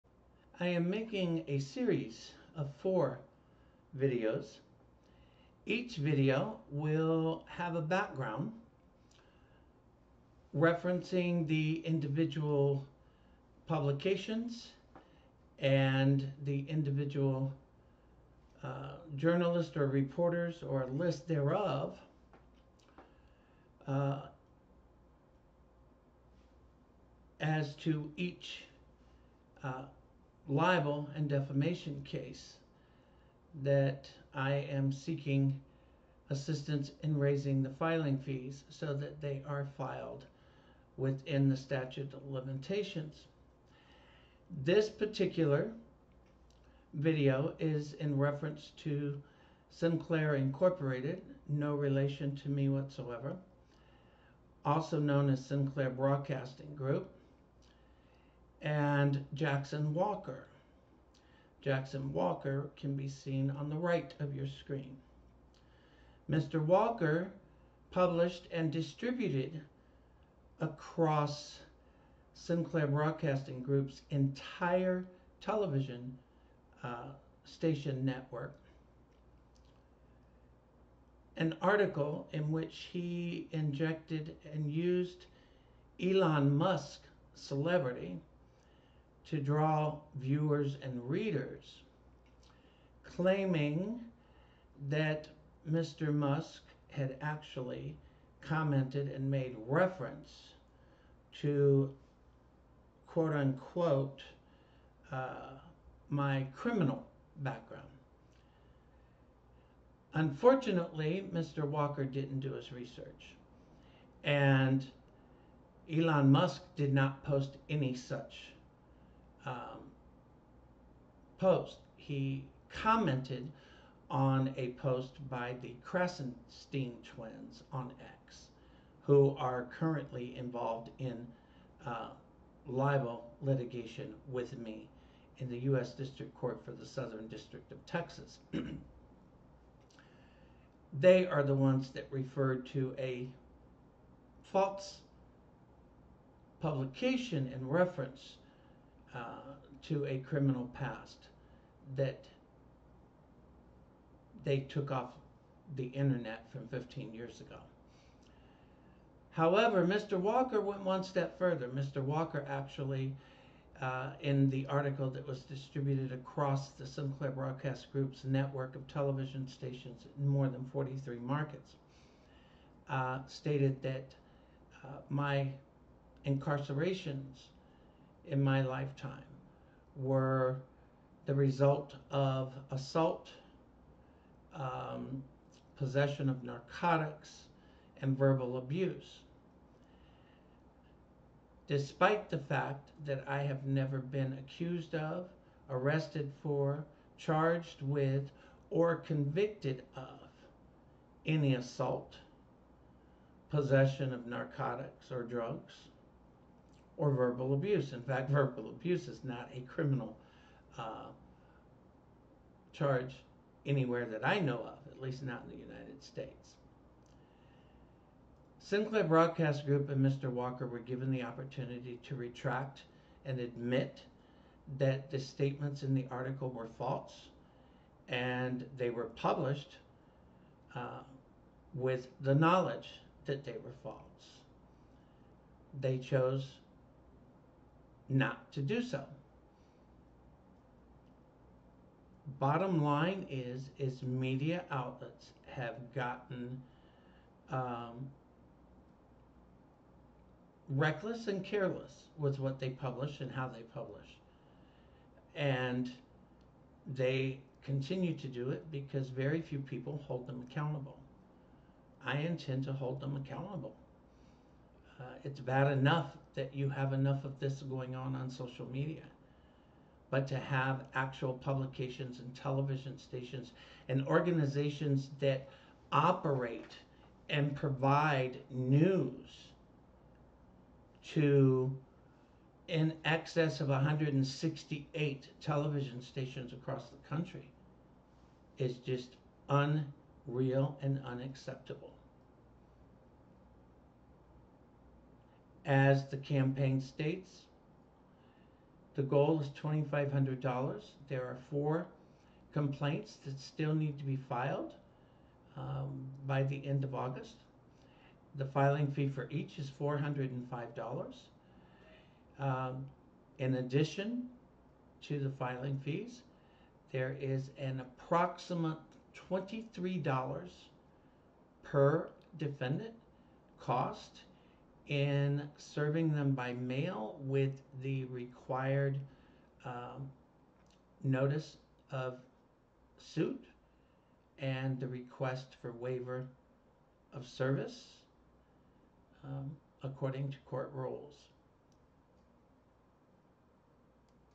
Sinclair Broadcast Group/ Jackson Walker Libel
via XSplit This is one of four short videos referring to each Libel Complaint being filed in campaign to raise filing fee funds to ensure those individual publications & journalists are held accountable.